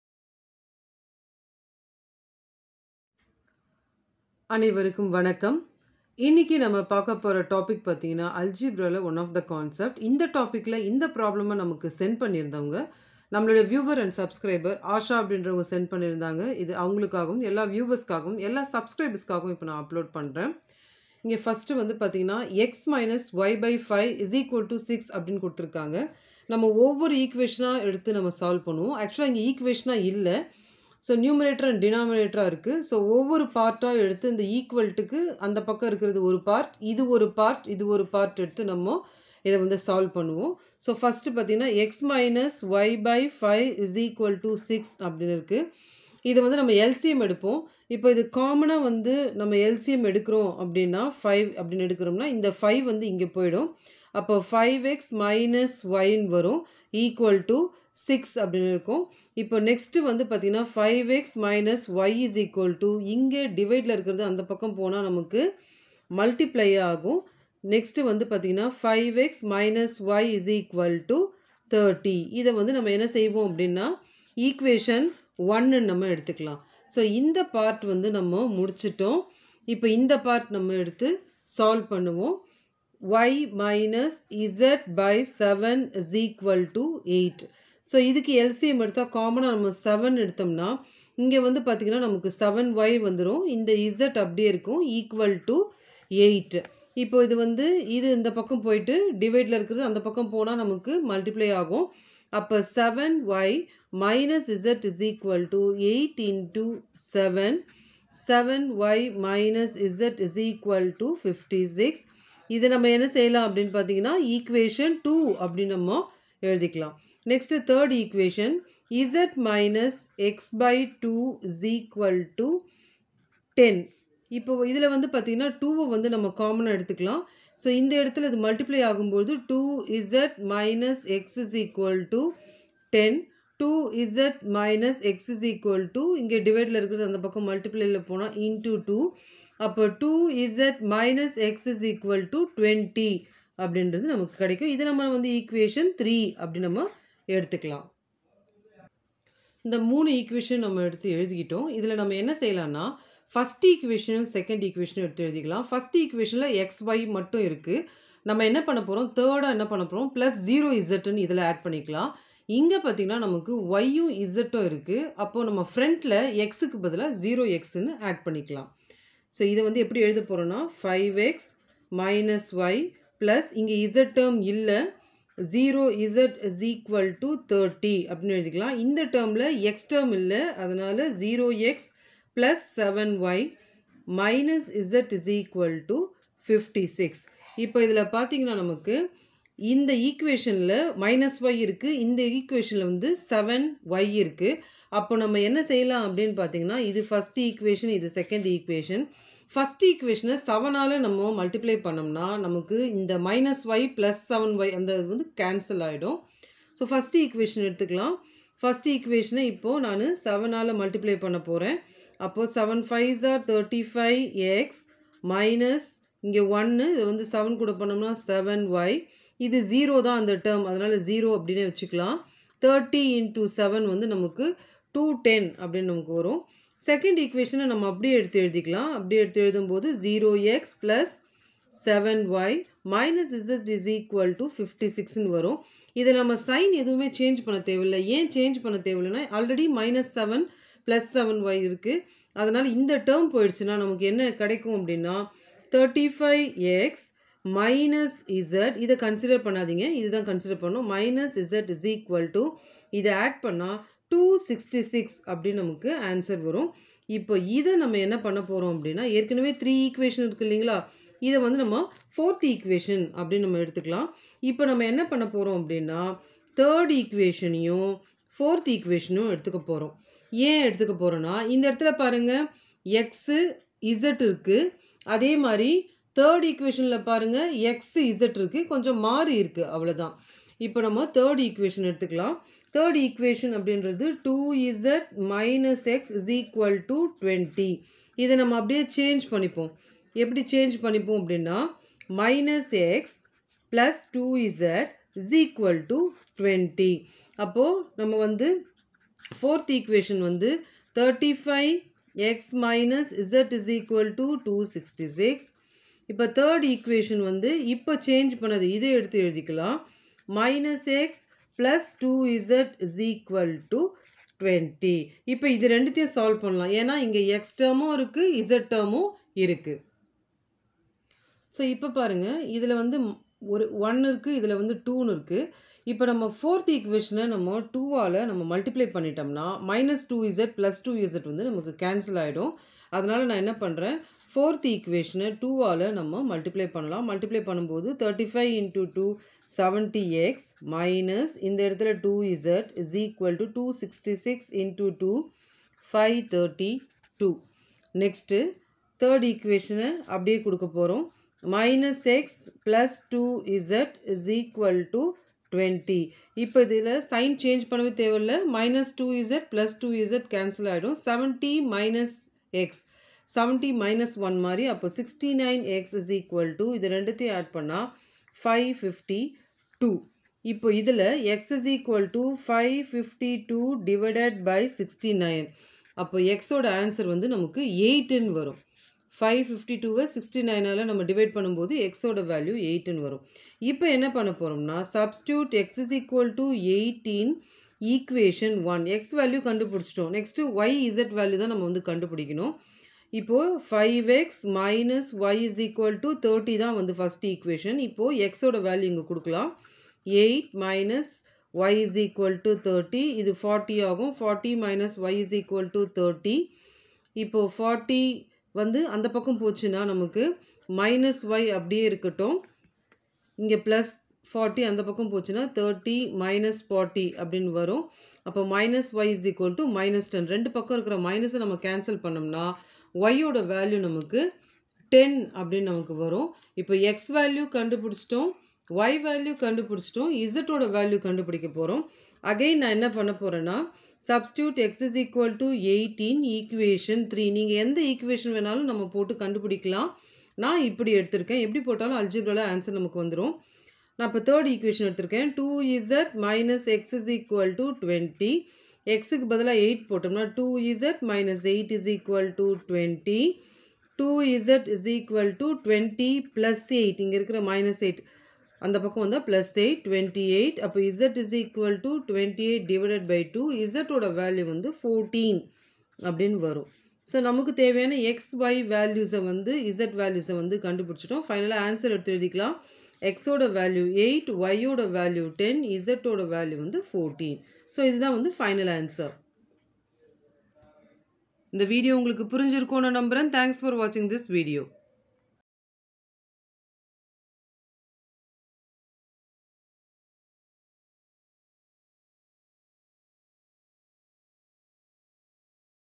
Algebra | Solve x-(y/5)=6,y-(z/7)=8,z-(x/2)=10 | part 6
Algebra part 6 is explained